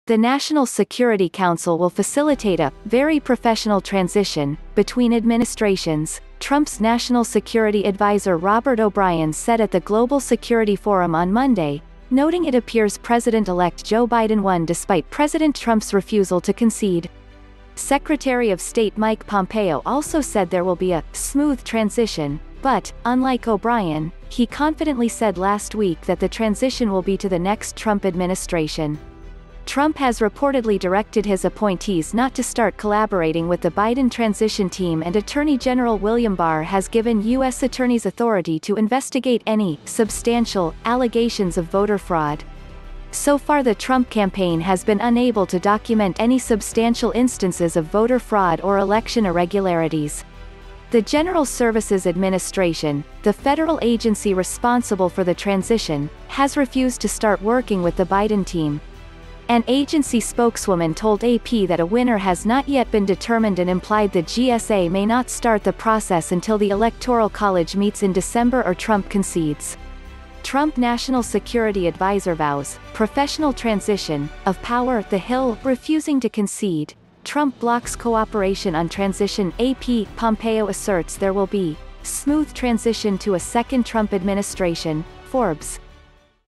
Trump’s National Security Advisor Says ‘Professional Transition’ Will
The National Security Council will facilitate a “very professional transition” between administrations, Trump’s National Security Advisor Robert O’Brien said at the Global Security Forum on Monday, noting it appears President-elect Joe Biden won despite President Trump’s refusal to concede. Secretary of State Mike Pompeo also said there will be a “smooth transition” but, unlike O’Brien, he confidently said last week that the transition will be to the next Trump administration. Trump has reportedly directed his appointees not to start collaborating with the Biden transition team and Attorney General William Barr has given U. S. Attorneys authority to investigate any “substantial” allegations of voter fraud. So far the Trump campaign has been unable to document any substantial instances of voter fraud or election irregularities. The General Services Administration, the federal agency responsible for the transition, has refused to start working with the Biden team. An agency spokeswoman told AP that a winner has not yet been determined and implied the GSA may not start the process until the Electoral College meets in December or Trump concedes. Trump national security adviser vows 'professional transition' of power (The Hill)Refusing to concede, Trump blocks cooperation on transition (AP)Pompeo Asserts There Will Be ‘Smooth Transition To A Second Trump Administration’ (Forbes)